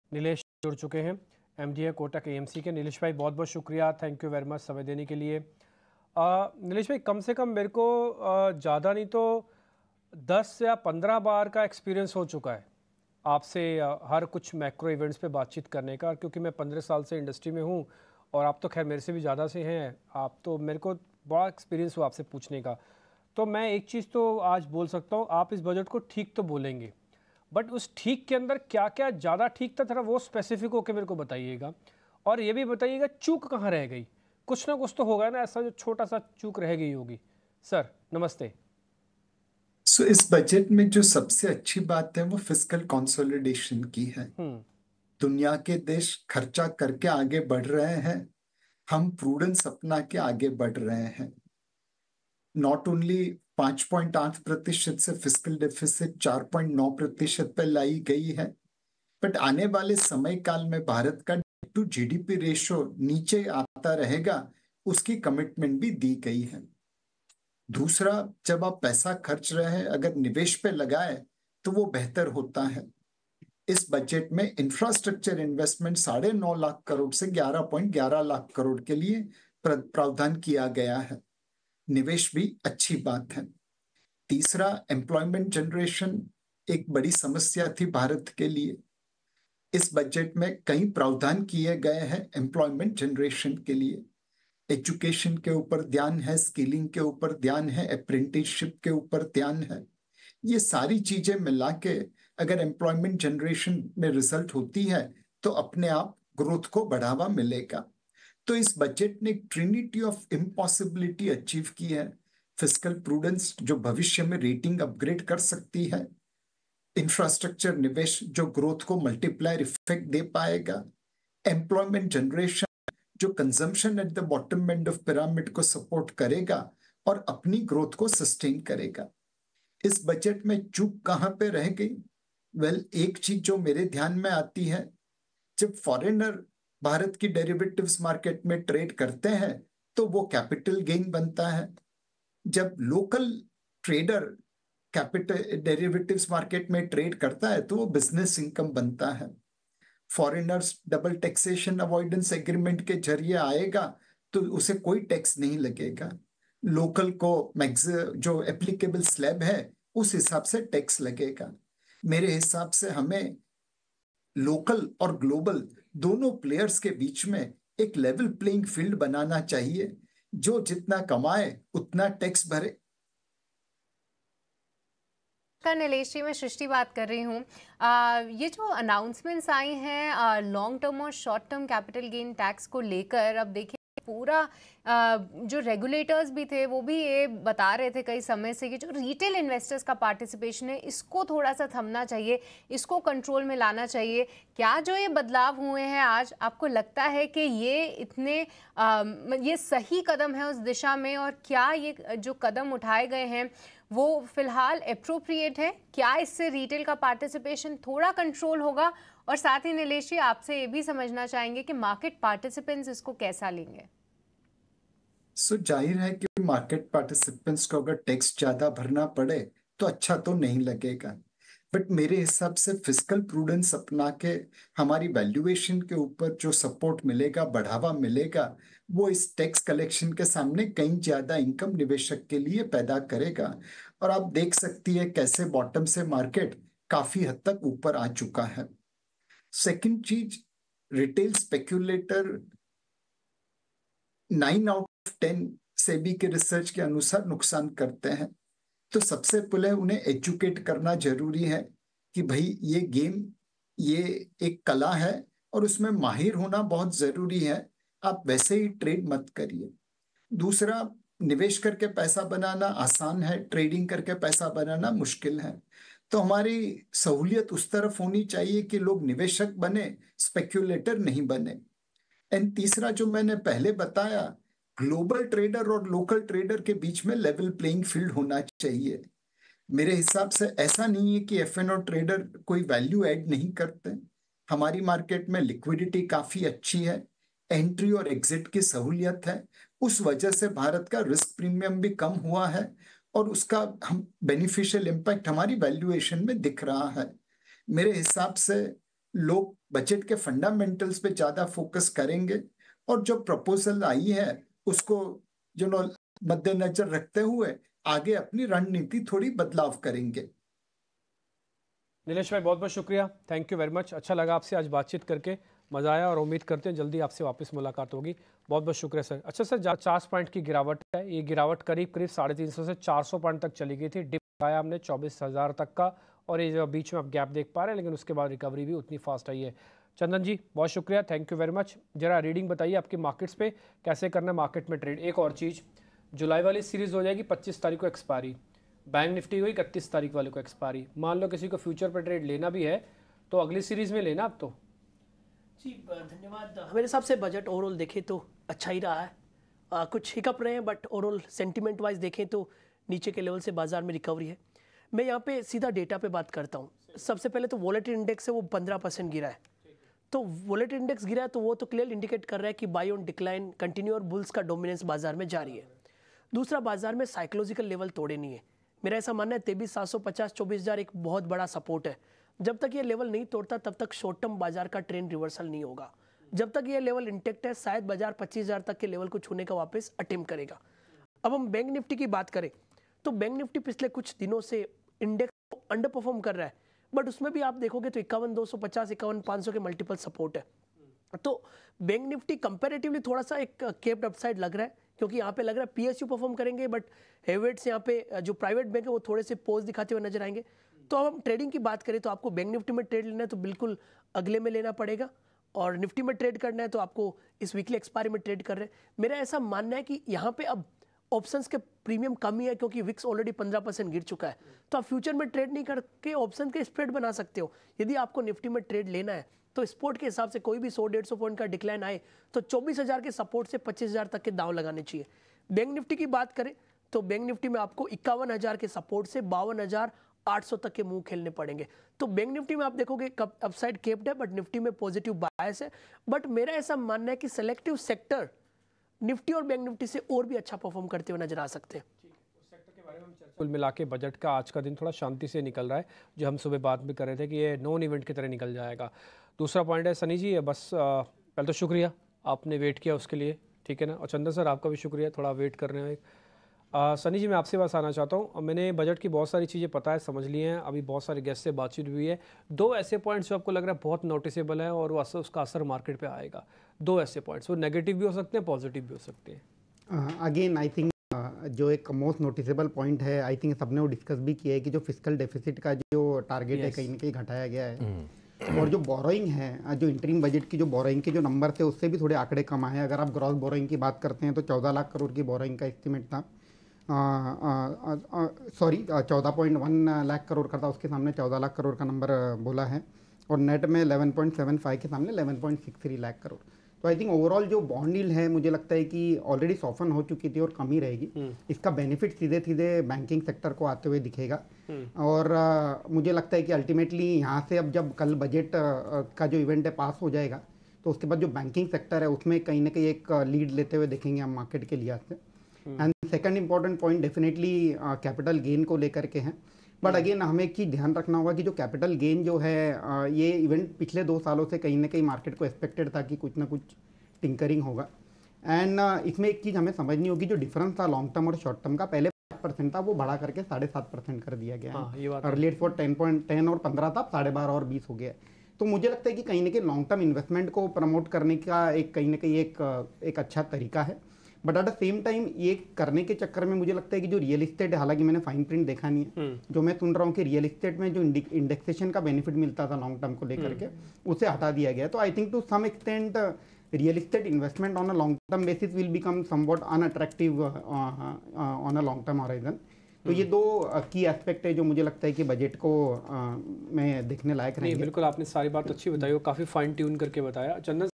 बजट-2024 पर Kotak MF के MD Nilesh Shah का नजरिया, Budget के किन ऐलानों से बाजार को होगा फायदा?
Budget Impact on Share Market : बजट-2024 पर Kotak MF के MD, नीलेश शाह (Nilesh Shah) का नजरिया, जानिए बजट में क्या अच्छा, किन ऐलानों ने किया निराश? स्टॉक मार्केट के निवेशकों के लिहाज से कैसा रहा बजट?